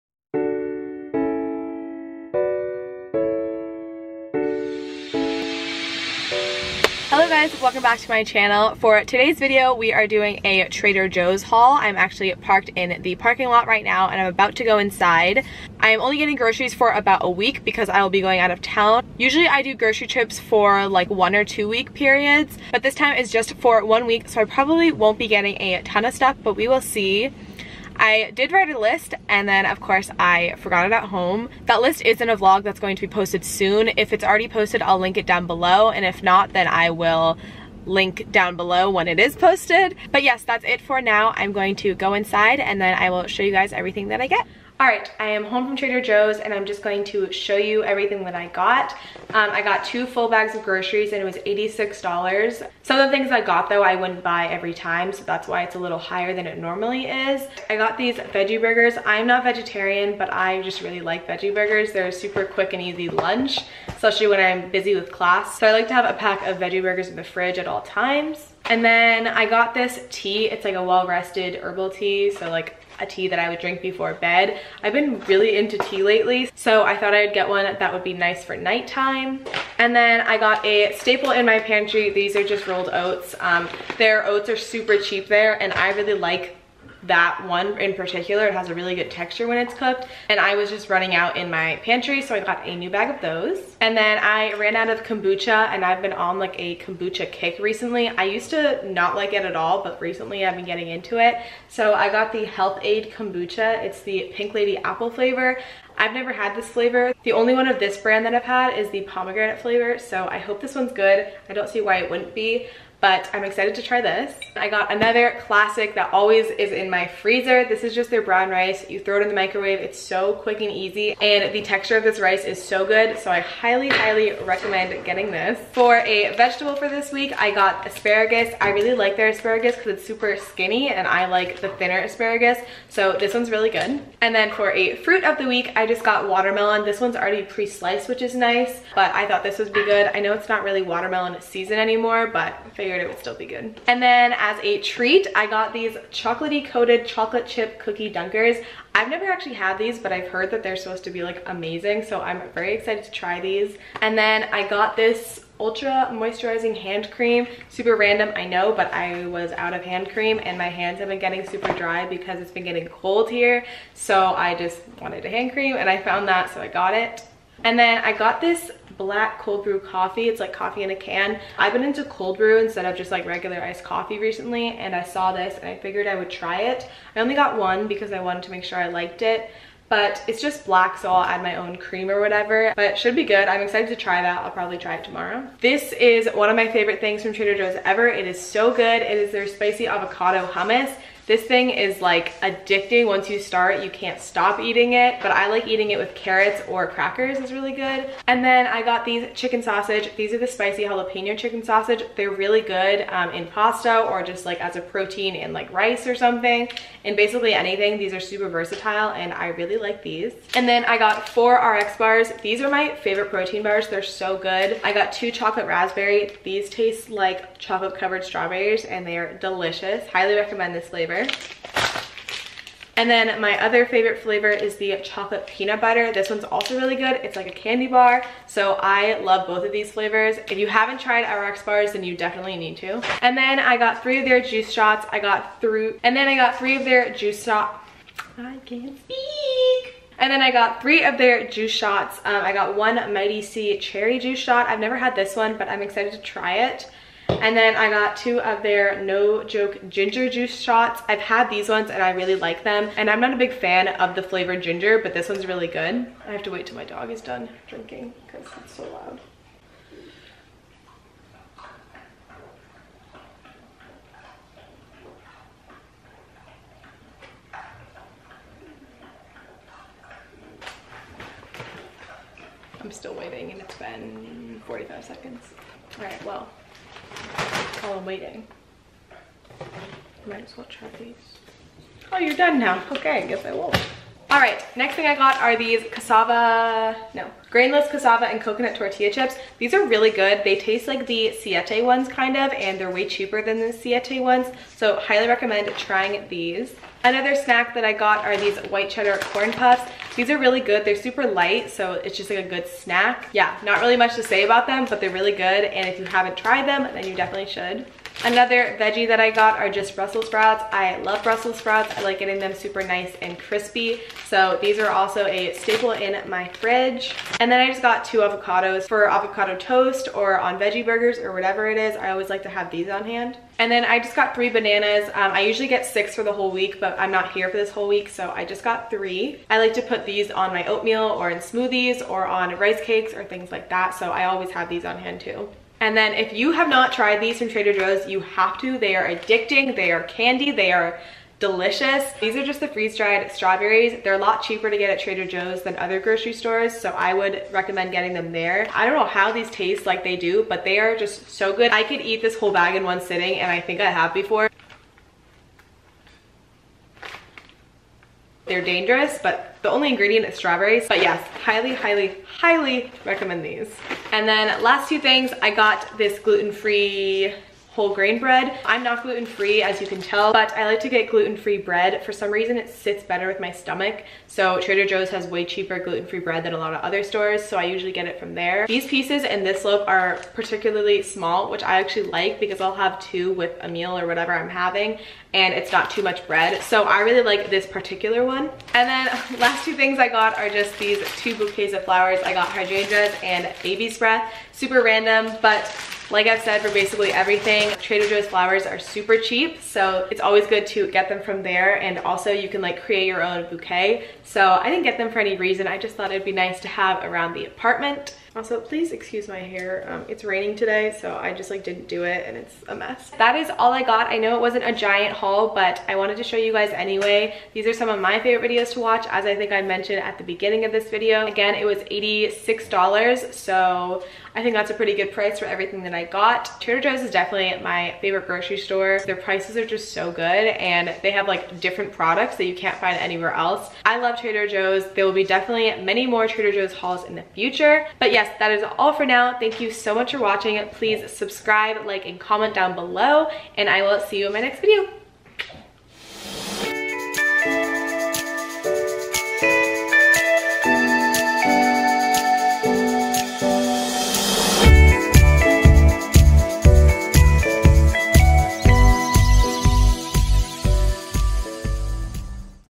trader joes grocery haul // balanced, healthy, and realistic!
hi guys!! in today's video, i show you a haul of the groceries that i picked up from Trader Joes! i get a good mix of panty/fridge staples, some fun snacks, and new things to try! just remember to eat what makes your body feel good :) subscribe, turn on the bell, and don't forget to give this video a thumbs up! let me know what you want to see next! XO